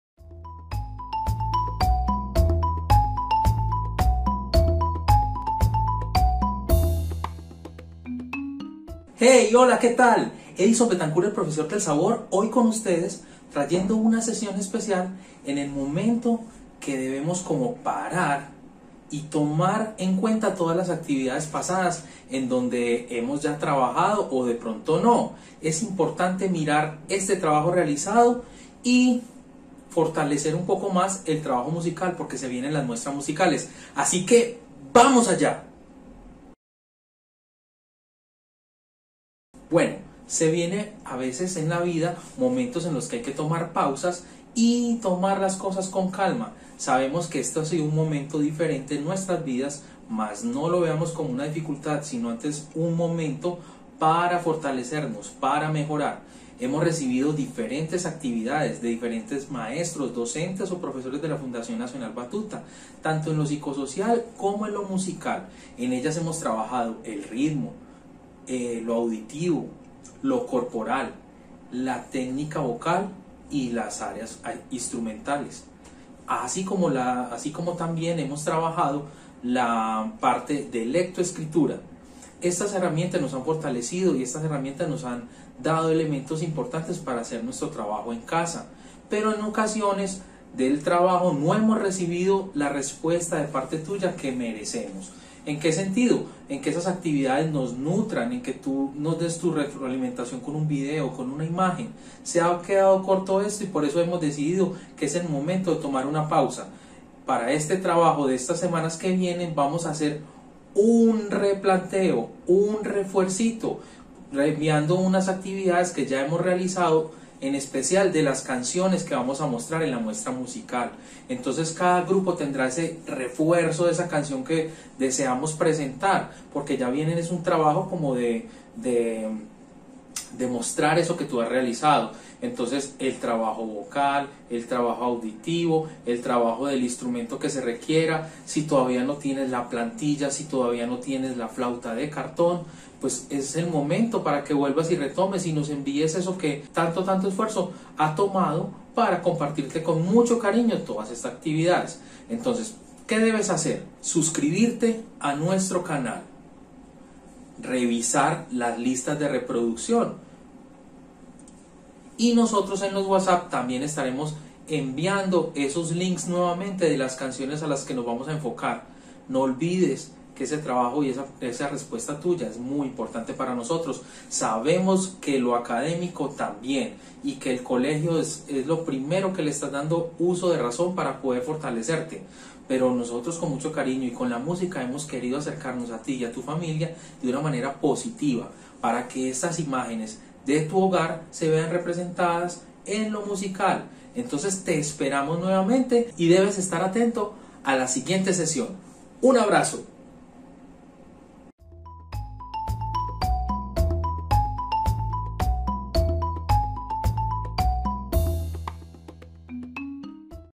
Formación Virtual-FNB-Resumen de Actividades
La música requiere una práctica constante, continua, como no estamos desde la presencialidad se hace necesario hacer una pausa y retomar varias de las sesiones que hemos compartido anteriormente. Con este vídeo queremos motivar las retroalimentaciones pero también queremos optimizar los resultados de aprendizaje para visualizarlos en las muestras de septiembre y noviembre.